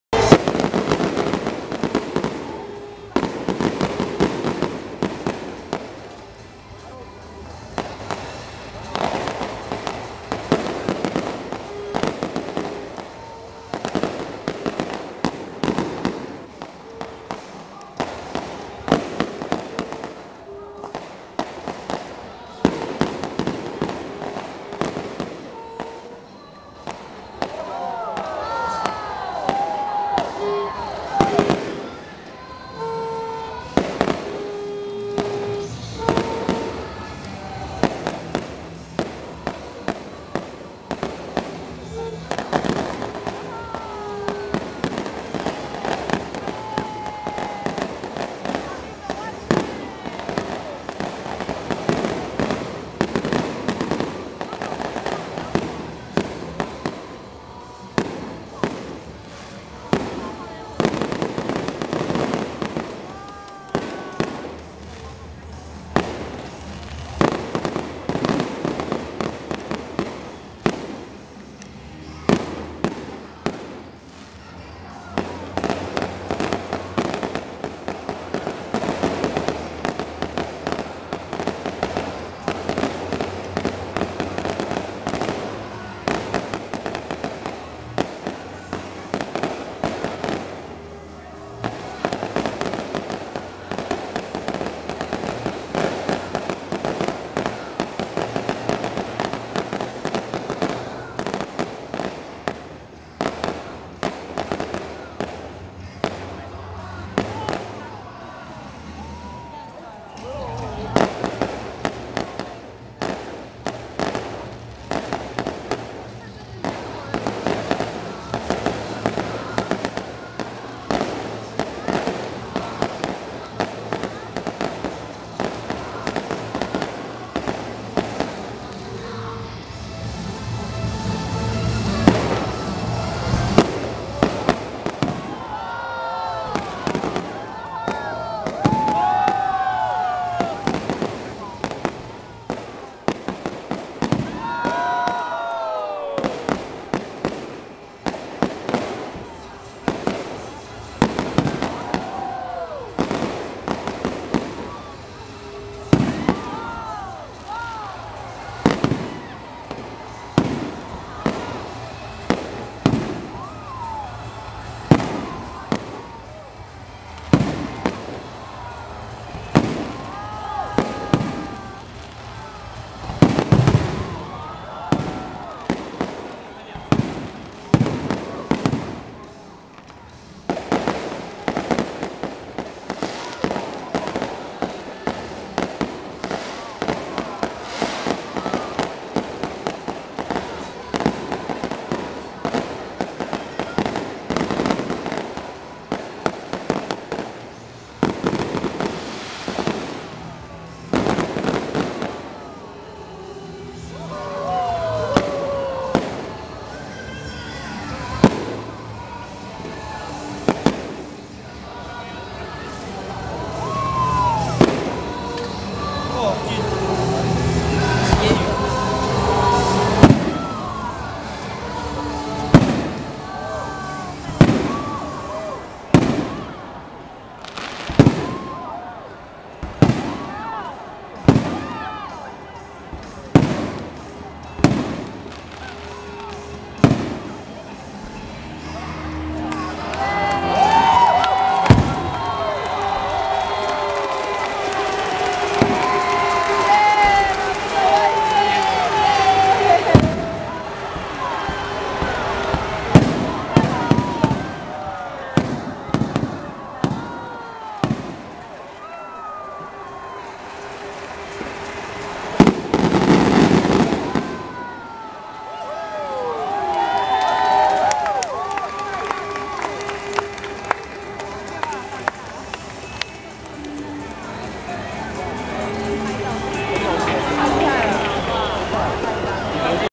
2013 CPBL Opening Day Fireworks/中華職棒開幕戰煙火秀
The opening day of the 24th season in Taiwan's professional baseball league's post game firework show, Lamigo Monkeys (Lamigo桃猿隊) beat Uni-President 7-11 Lions (統一7-ELEVEn獅隊) 10 - 8 at Taoyuan International Baseball Stadium (桃園國際棒球場). Ilha Formosa, Taiwan Will Touch Your Heart.